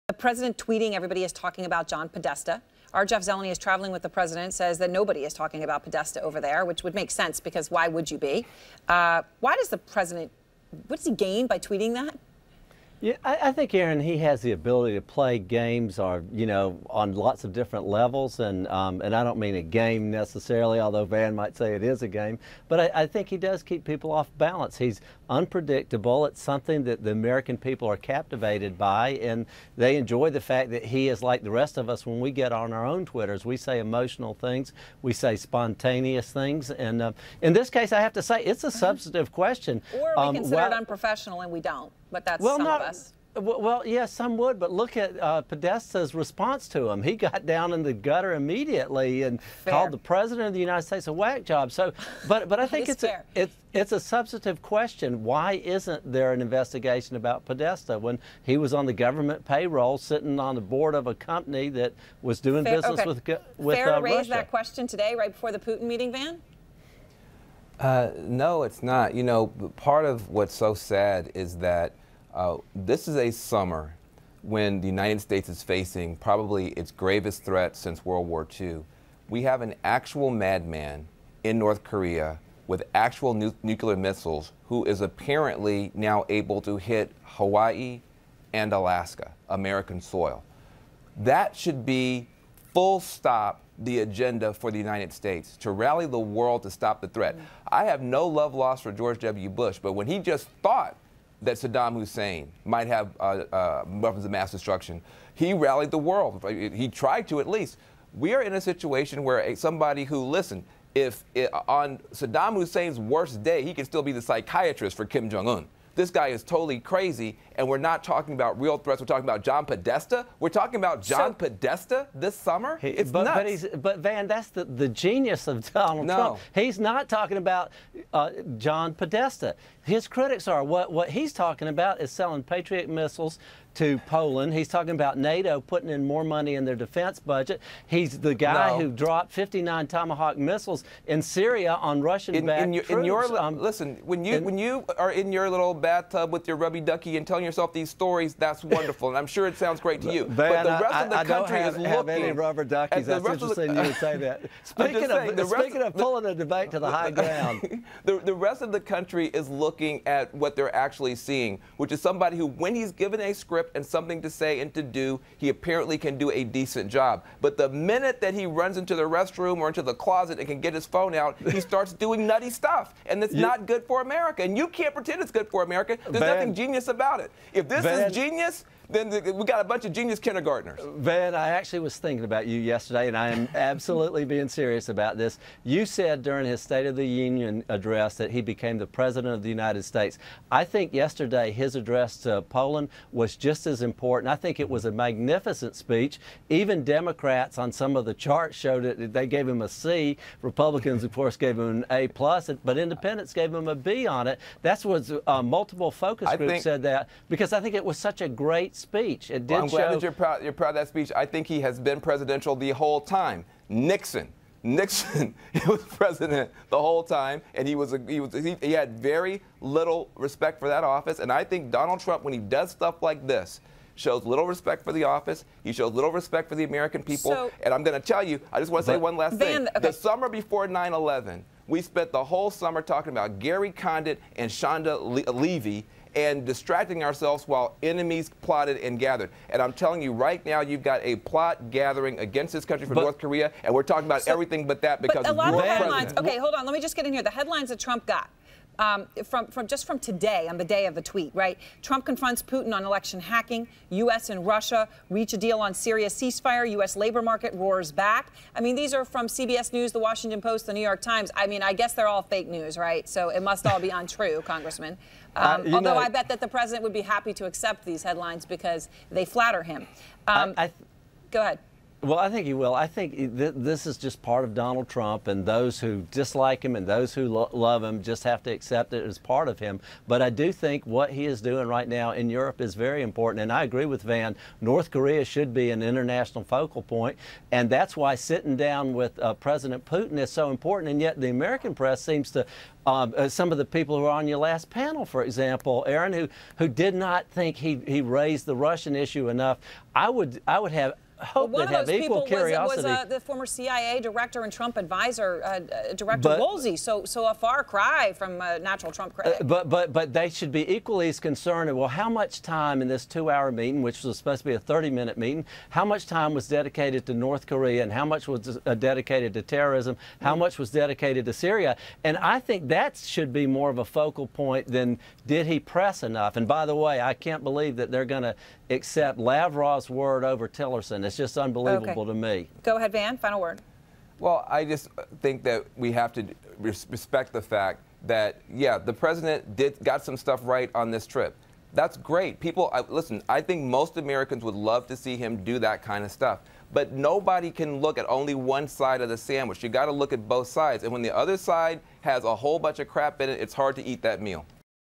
Van Jones: Trump should talking North Korea, not Podesta
CNN's Van Jones tells former Trump adviser Jack Kingston "it's nuts" that President Donald Trump, while attending the G20 summit, raised concerns over John Podesta and not Kim Jong Un.